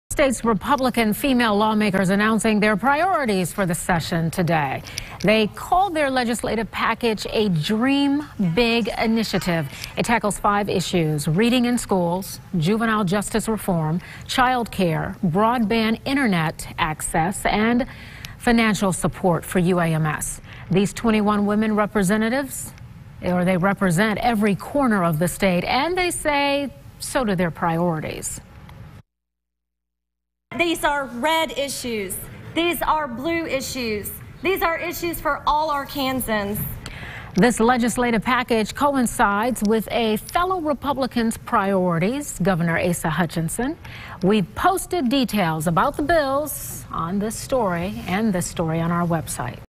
The Dream Big Initiative
The State's Republican female lawmakers announcing their priorities for the session today.